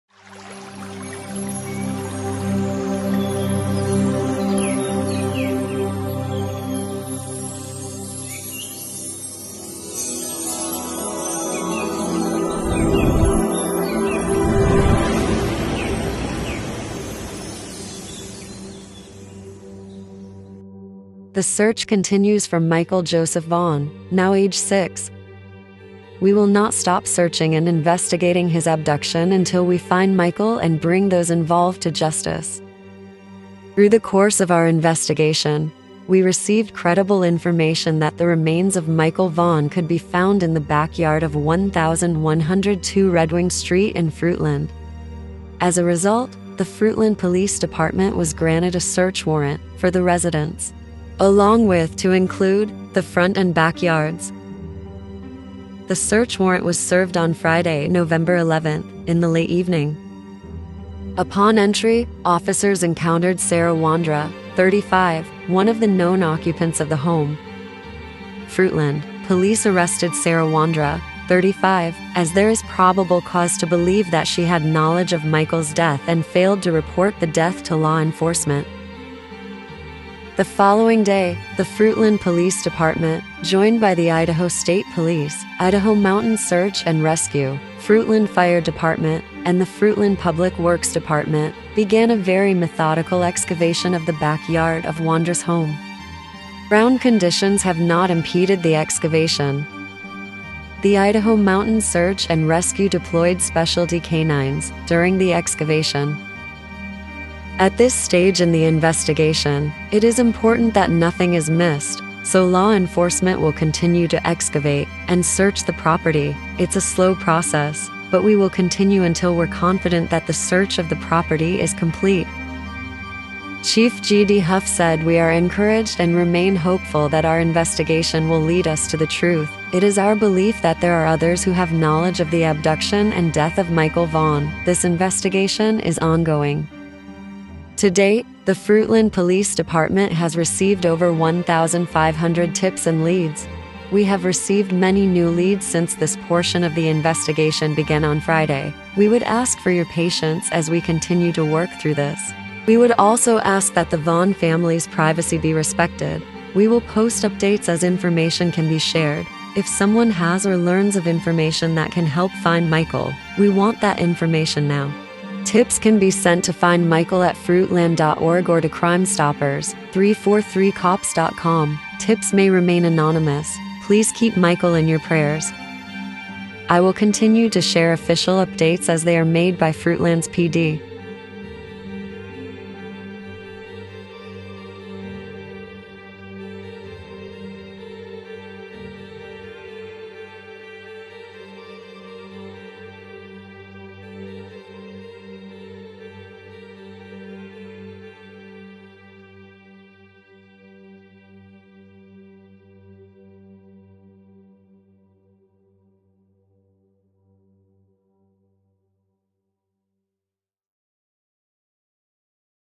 Missing: Michael Monkey Vaughan | Full, Official Statement Released By City Of Fruitland Police
Missing: Michael Monkey Vaughan | Full Interview With JD Huff | Improved Audio & Subtitles

Taken from a recent interview - not from today

Monkey went missing in his neighborhood the evening of Tuesday, July 27, 2021.

Investigators and locals have been on the search ever since for Vaughan, who is described as 43 inches tall and weighing 50 pounds.

Michael.  He was last seen wearing a blue Minecraft t-shirt with dark blue boxer briefs. He answers to the nickname of 'Monkey' and lives near the corner of SW 9th St and Arizona in Fruitland.

BOISE, Idaho — Michael Joseph Vaughan, a bright and energetic 5-year- old was last seen on July 27 near his home in Fruitland, Idaho. Fruitland Police, aided by more than a dozen agencies and the FBI, have searched for more than 2,500 hours, including with the use of drones and search dogs

Google

City of Fruitland P.D. - Statement